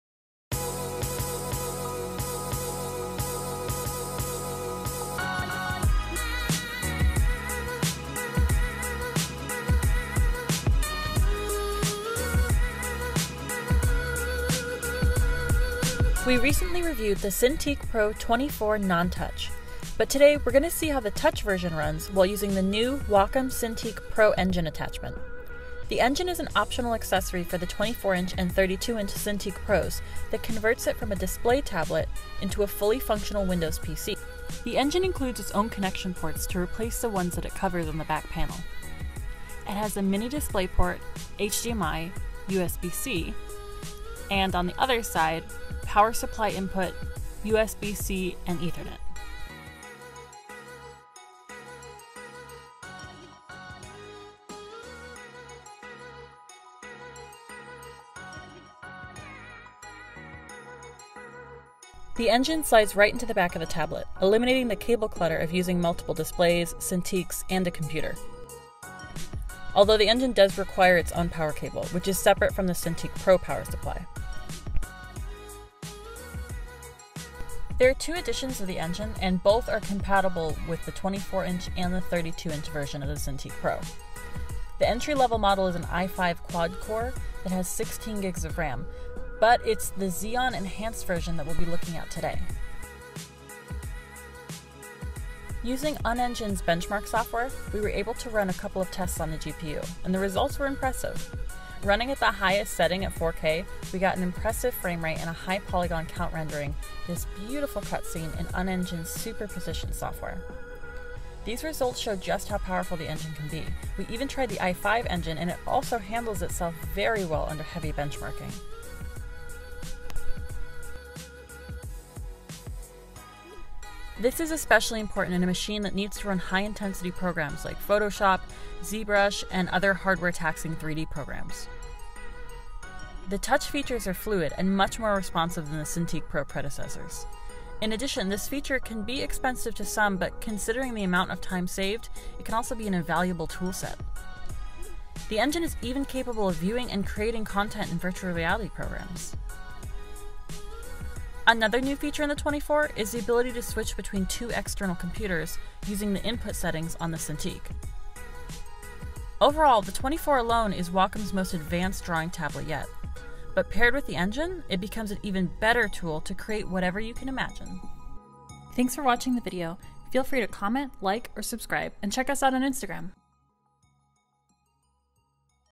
Wacom Cintiq Pro 24 Touch and Xeon Engine Overview
We put the Wacom Cintiq Pro 24 Touch and the Xeon Engine under rigorous testing.

Let us know if you have one and how you like it!

Featuring Wacom Products like Cintiqs, Tablets, MobileStudio's, and accessories.